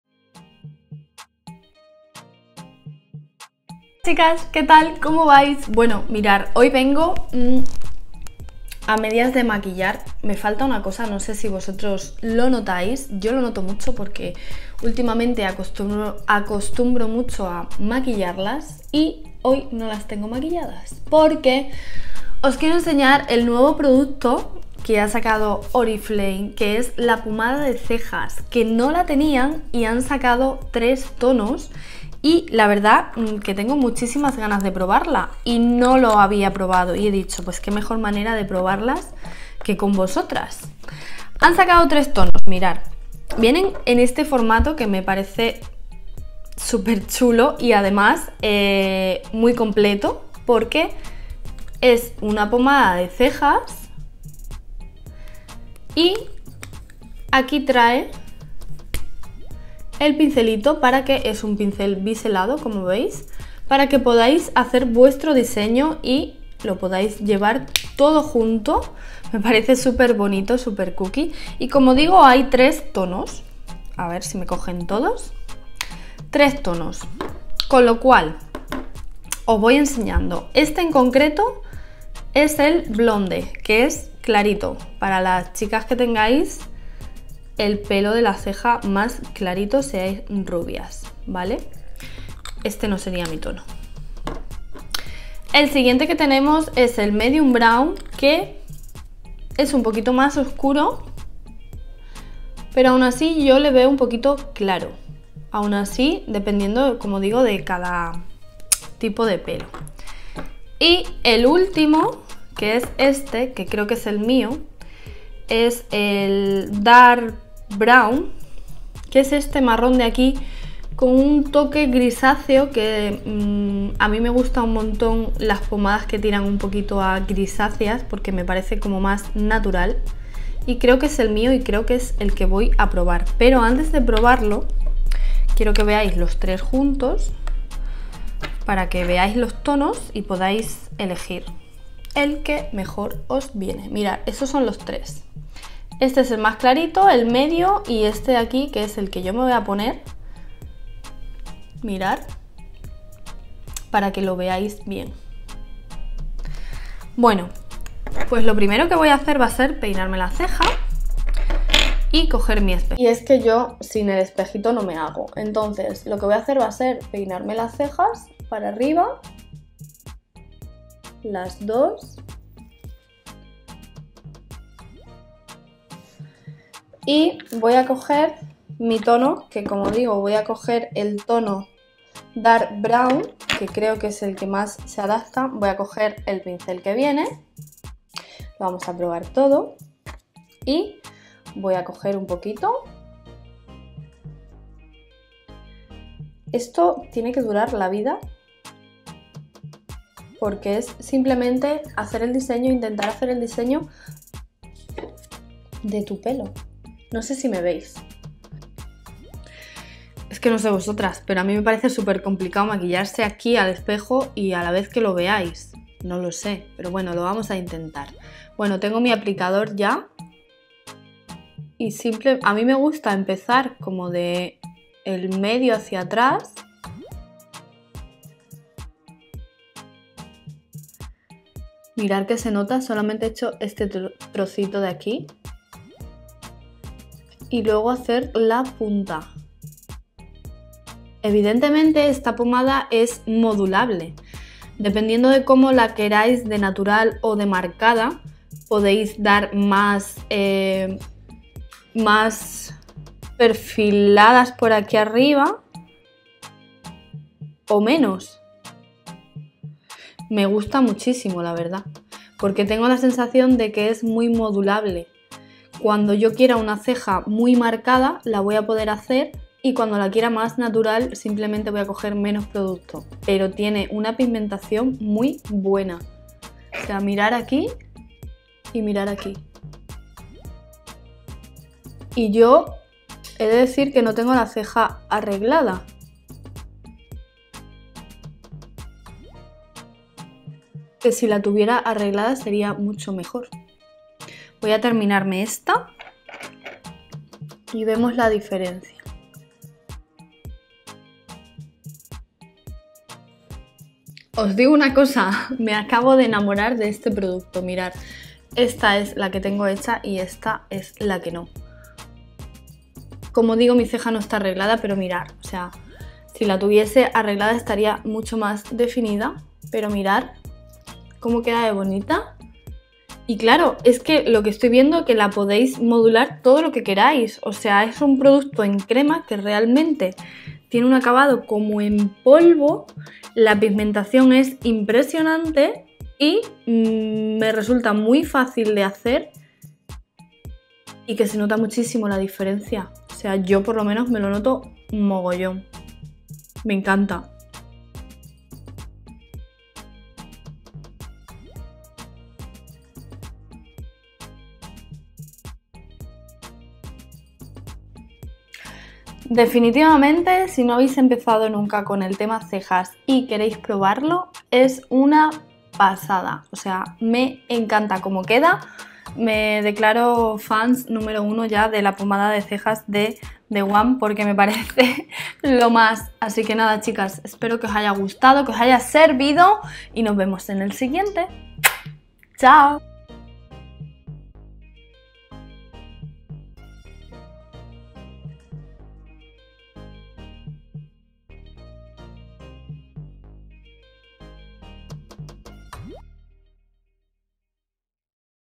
DISEÑA TUS CEJAS CON LA NUEVA POMADA THE ONE I ORIFLAME ESPAÑA
Descubre la nueva Pomada para Cejas the ONE en 3 tonos. Su textura crema se transforma en polvo sobre tu ceja, por lo que podrás elegir o bien, un look super definido o bien algo más natural rellenando las calvitas para un maquillaje efecto cara lavada.

¡Es súper fácil de utilizar! Descubre cómo hacerlo en este vídeo de nuestra Autumn Make Up School de la mano de la embajadora The ONE Briyilia Pérez.